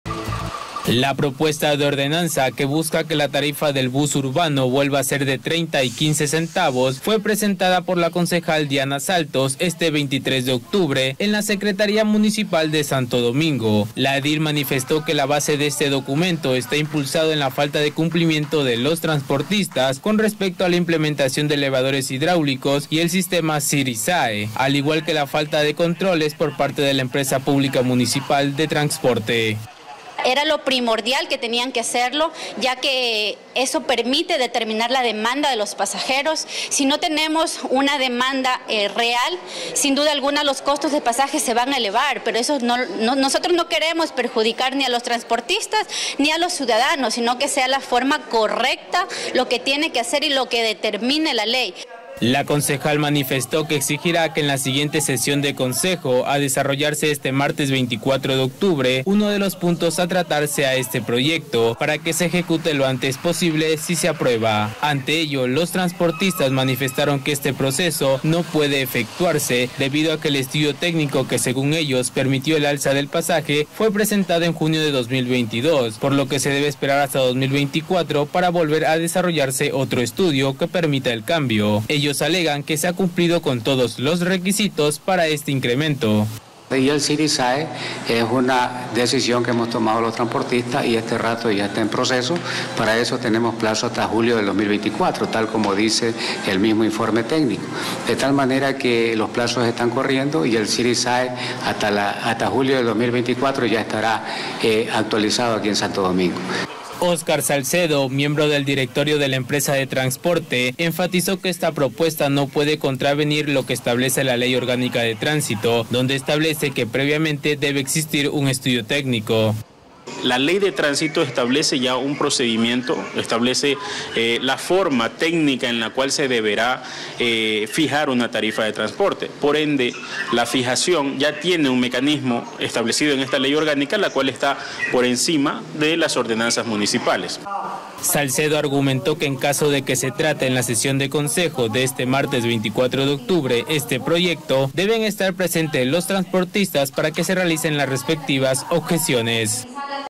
Diana Saltos presentó una propuesta de ordenanza ante el alza de los pasajes en Santo Domingo
La concejal Diana Saltos presentó una propuesta de ordenanza ante el alza de los pasajes en Santo Domingo. Esta medida busca que la tarifa vuelva a ser de $0.30 y  $0.15 respectivamente. El gremio de transportistas descarta que este proceso se realice debido a que aún no han pasado dos años desde que se presentó el estudio técnico, el cual está respaldado por la ley de tránsito.